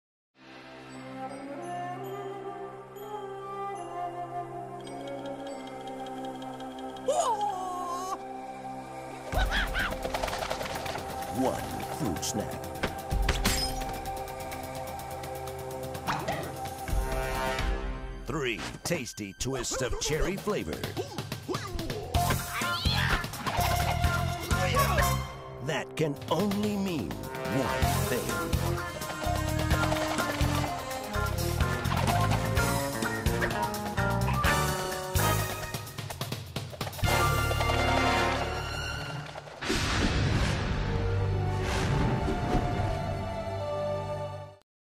Kellogg's Fruit Twistables - Twists of Fury (2005, USA)
The unedited version of a stop-mo "twist" on classic martial arts movies made for Fruit Twistables Cherry Explosion, based on characters first animated by Aardman. Directed by Paul Harrod at Bent Image Lab.